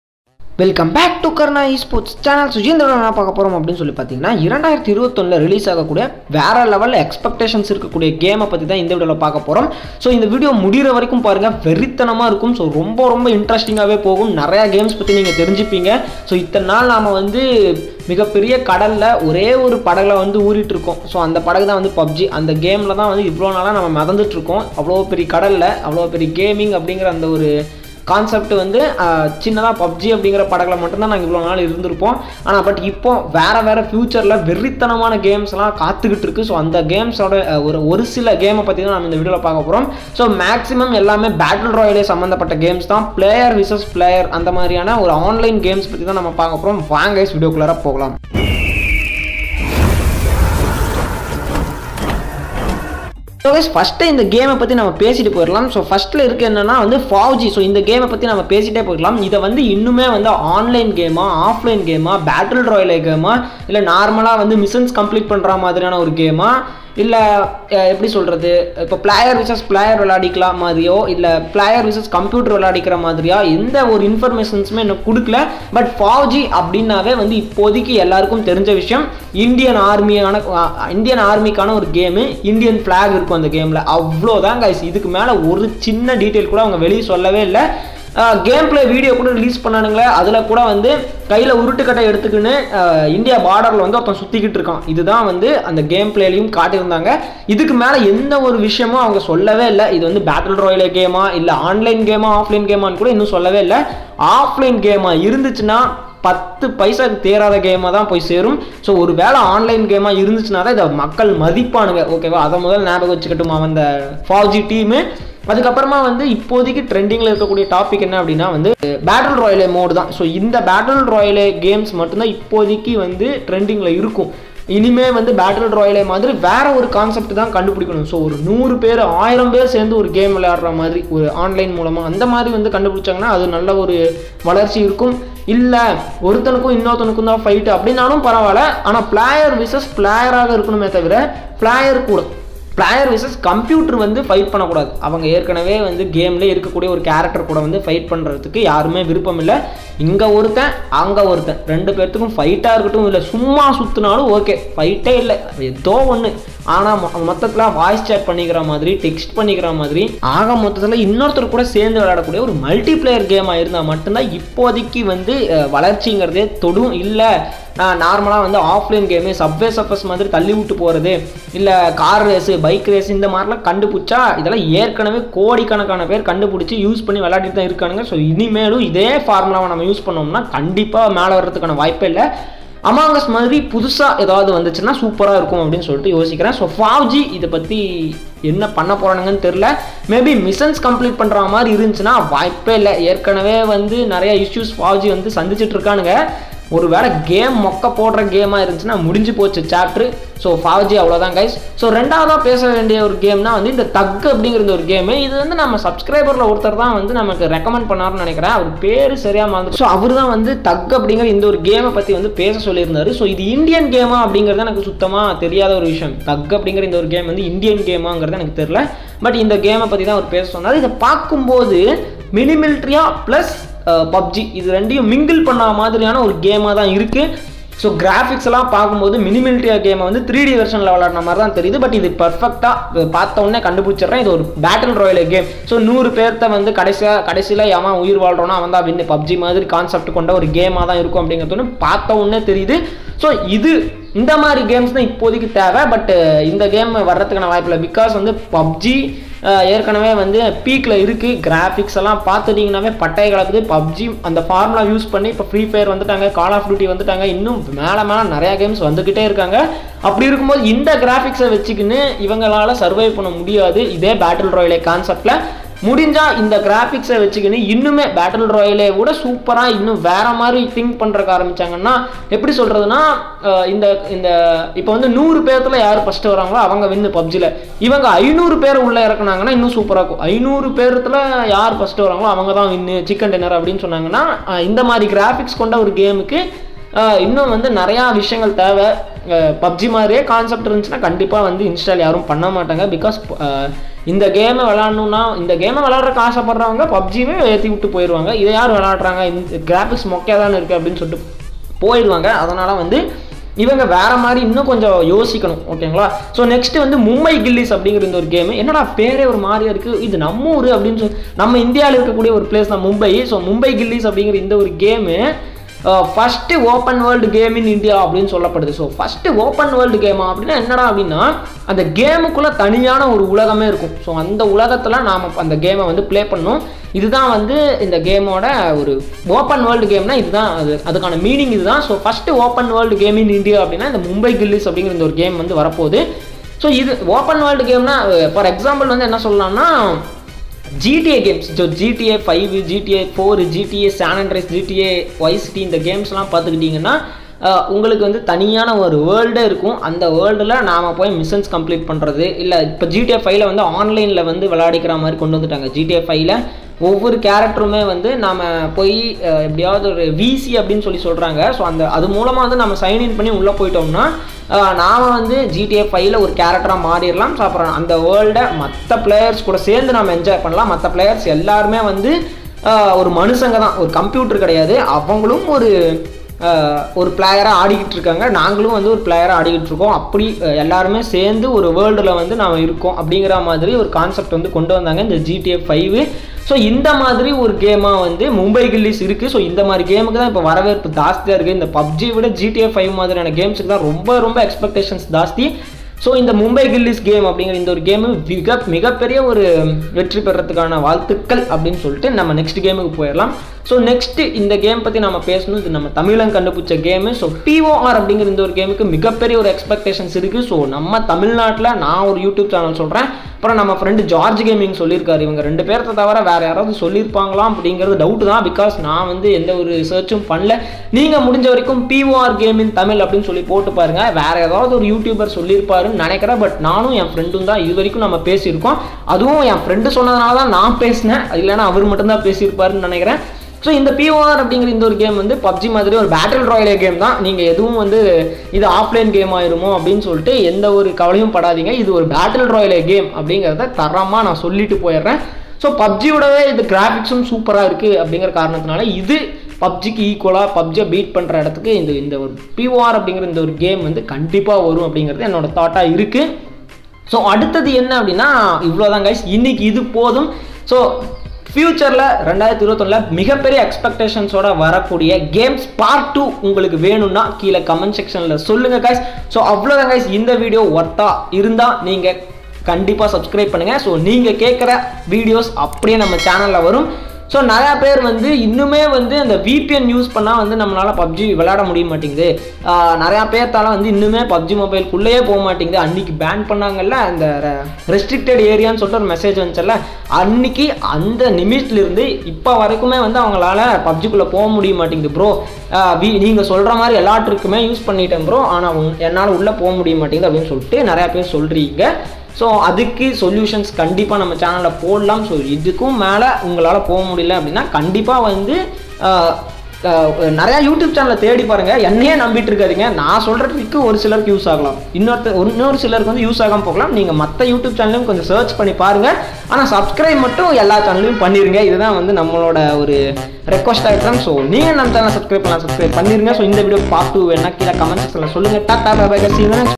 Upcoming indian games of 2021 in tamil/2021 indian games
Upcoming games of 2021 in tamil 2021 games in tamil many games are coming in 2021 with big expectation

1.FAUG
2.MUMBAI GULLIES
3.THUG
4.POR